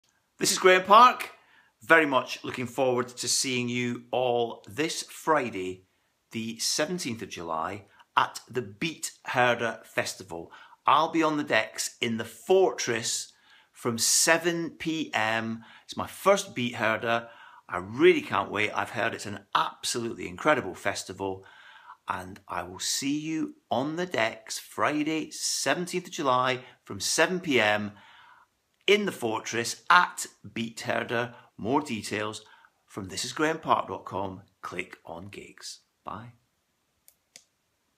This Is Graeme Park: Beat-Herder 17JUL15
Join me in The Fortress at Beat-Herder from 7pm Friday 17th July in deepest, darkest Lancashire. Looking forward to seeing you there!